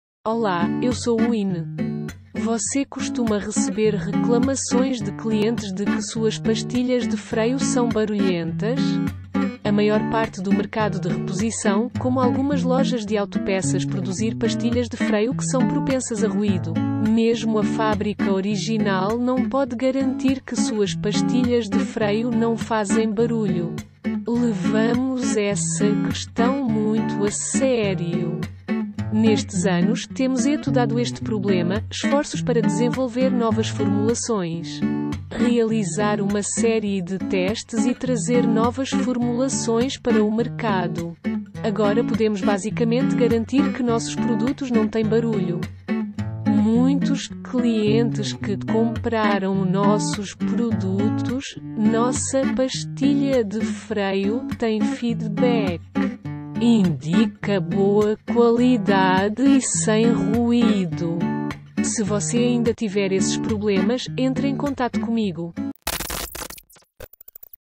Você costuma receber reclamações de clientes de que suas pastilhas de freio são barulhentas?
Problema de ruído da pastilha de freio!
A maior parte do mercado de reposição, como as pastilhas de freio produzidas por algumas lojas de autopeças, são propensas a ruídos,
Mesmo a fábrica original não pode garantir que suas pastilhas de freio sejam livres de ruído.
Levamos essa questão muito a sério!
Estamos trabalhando nesse problema de ruído há vários anos,
Esforços para desenvolver novas formulações,
realizar uma série de testes,
E colocar novas fórmulas no mercado.
Agora podemos basicamente garantir que nossos produtos são livres de ruído!
Muitos clientes que compraram nossos produtos também deram feedback,
Indica boa qualidade sem ruído.
Se você ainda tiver esses problemas, entre em contato comigo!